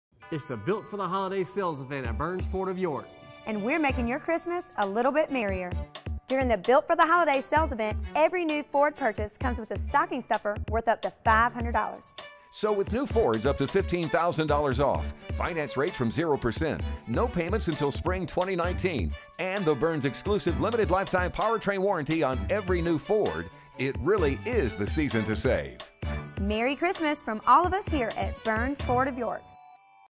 Season to Save at Burns Ford of York | BurnsFordofYork.com
It really is the Season to Save with up $15,000 Off MSRP on new Fords, 0% APR up to 72/mths and No Payments for 90 Days! Also, for a limited time with every new Ford purchase receive a stocking stuffer worth up to $500! Shop Burns Ford of York and BurnsFordofYork.com.

Burns Ford of York
1667 Old York Rd
York, SC 29745

BurnsFordofYork.com